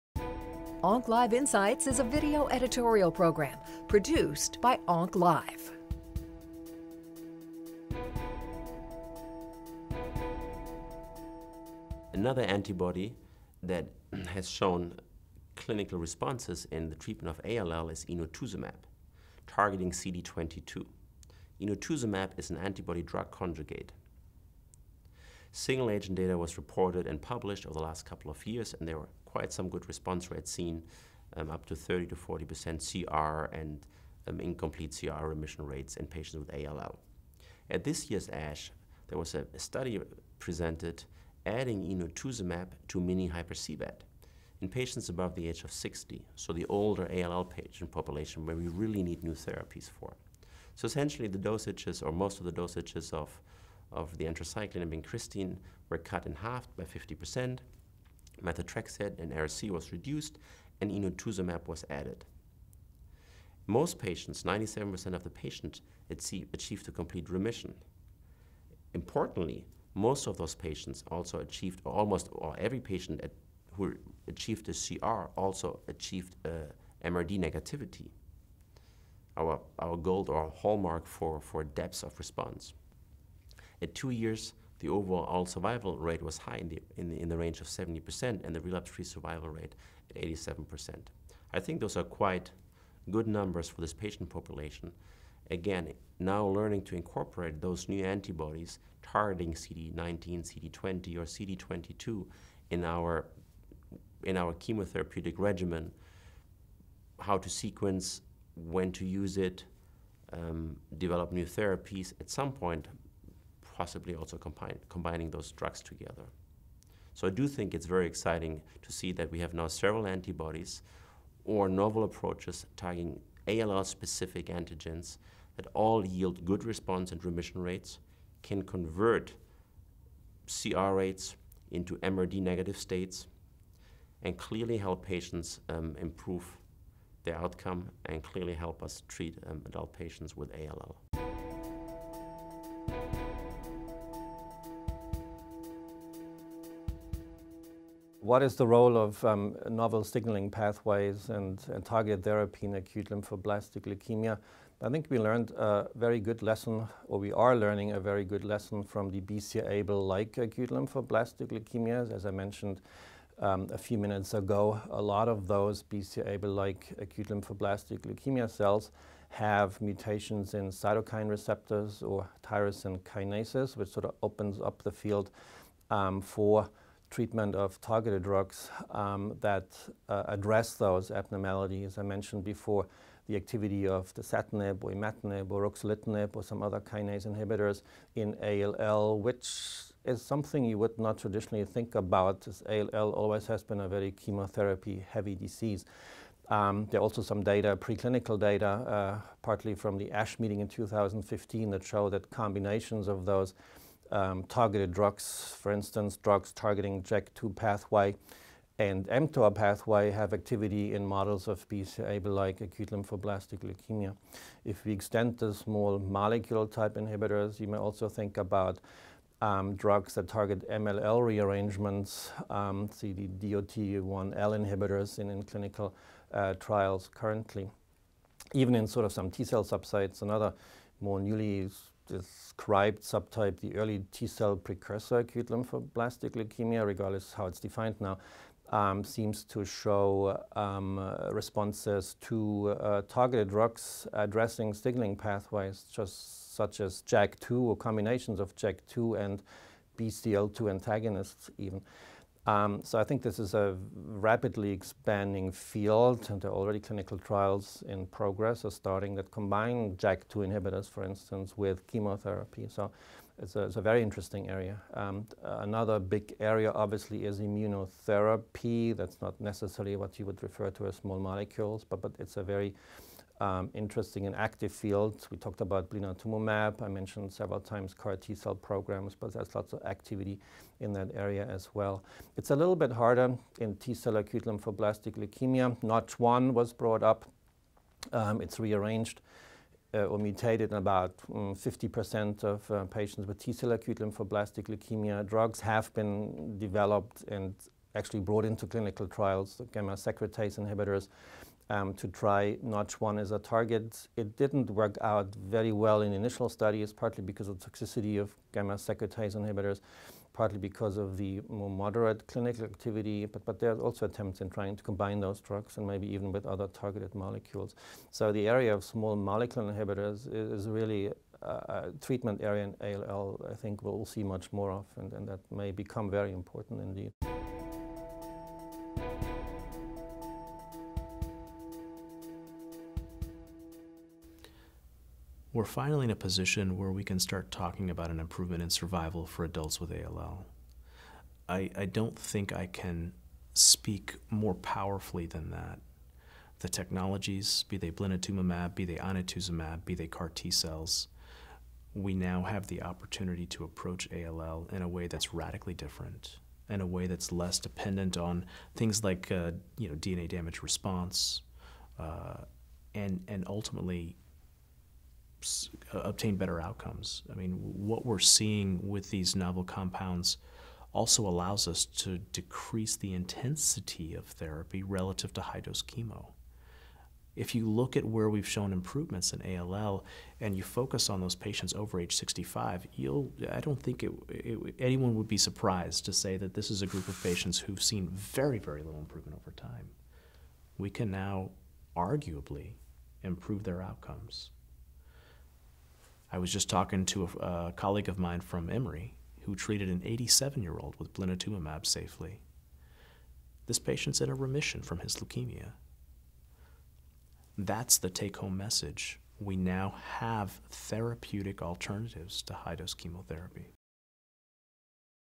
Emerging Therapeutic Alternatives in Acute Lymphoblastic Leukemia
The panelists, Raoul Tibes, MD, PhD, Stefan Faderl, MD, and Bijal D. Shah, MD, introduce emerging therapeutic alternatives to chemotherapy in the treatment of acute lymphoblastic leukemia.